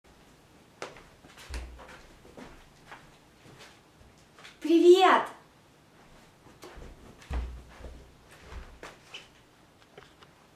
Privet
Just saying Hi in Russian to my family and friends